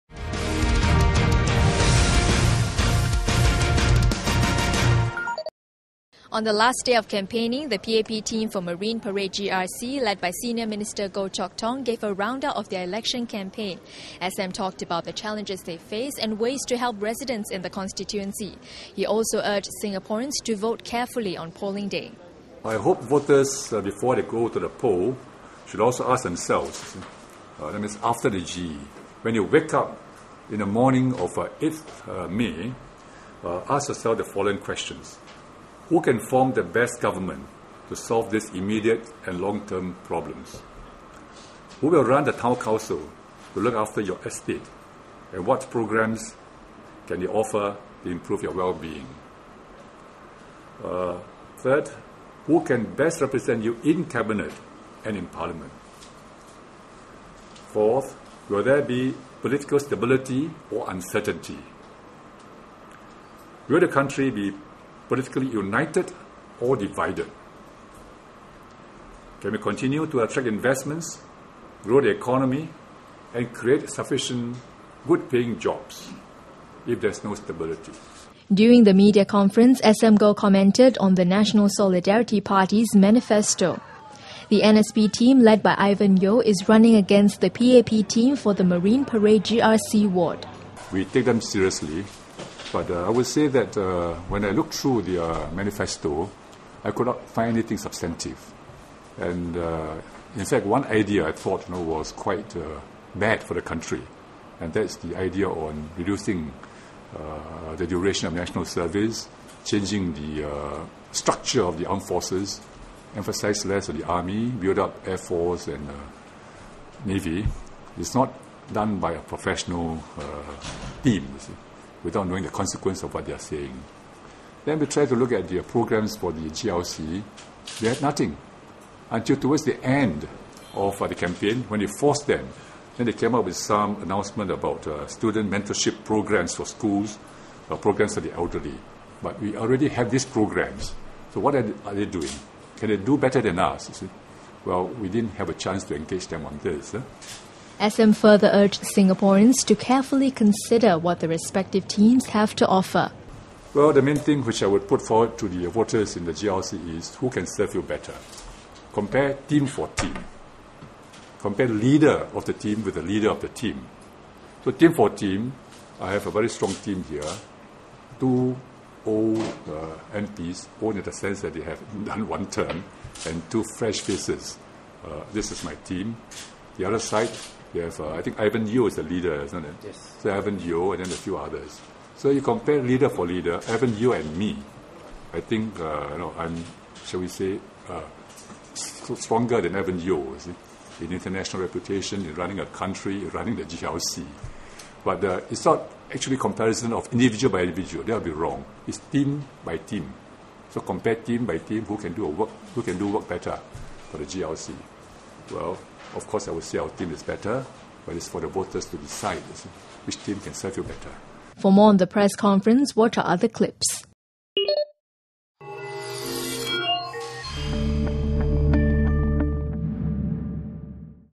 SM Goh: Vote for the team, not indivuduals
In his campaign round up, Senior Minister Goh Chok Tong asked voters to consider carefully what each team has to offer, instead of just looking at individual candidates. He also urged Singaporeans to think about Singapore's long term challenges such as education and healthcare, over short term problems like housing and immigration.